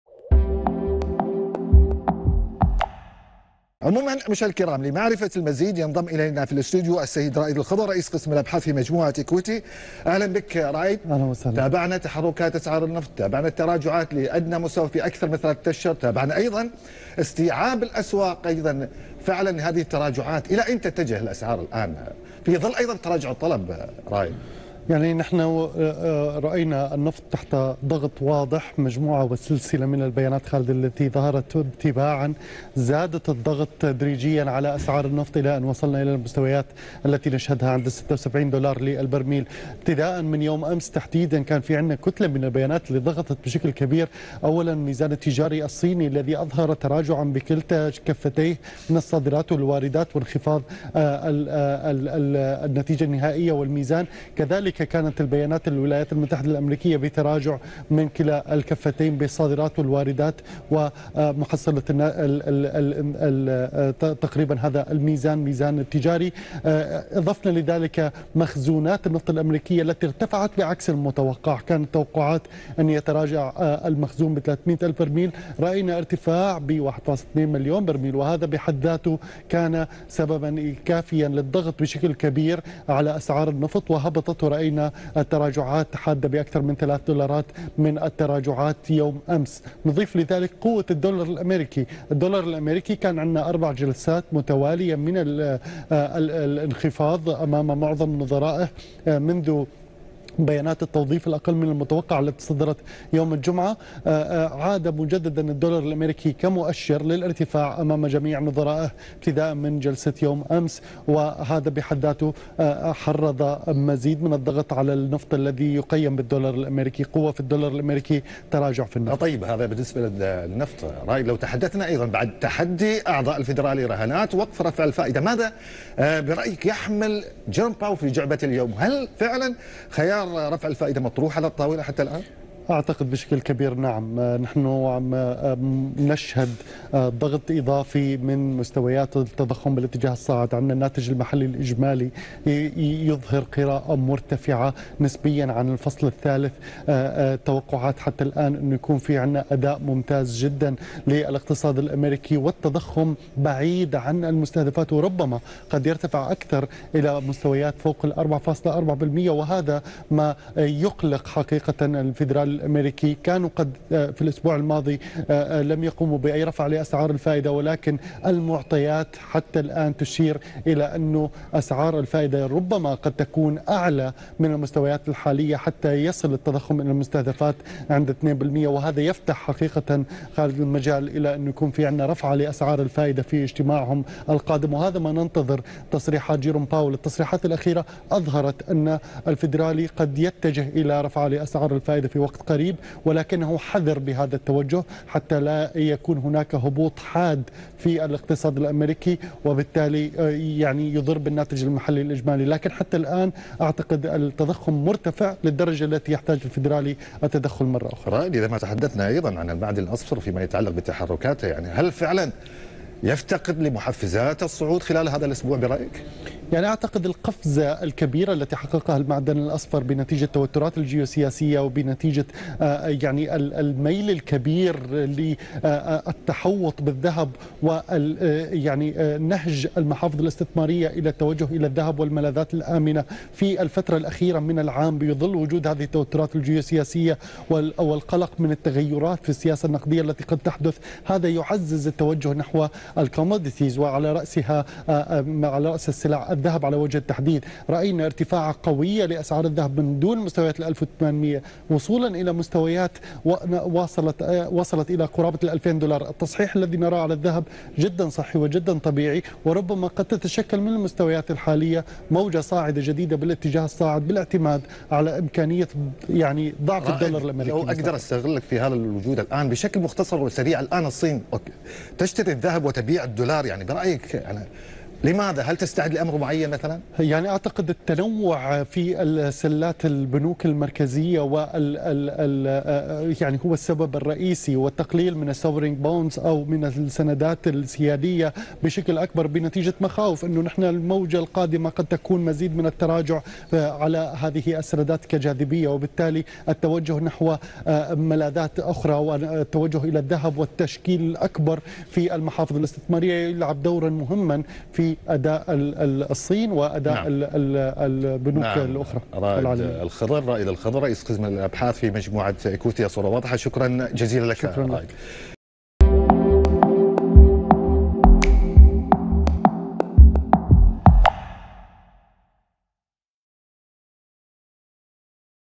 النفط يتراجع عند أدنى مستوى له في ثلاثة أشهر
نظرة حول تحركات النفط والمعدن الثمين.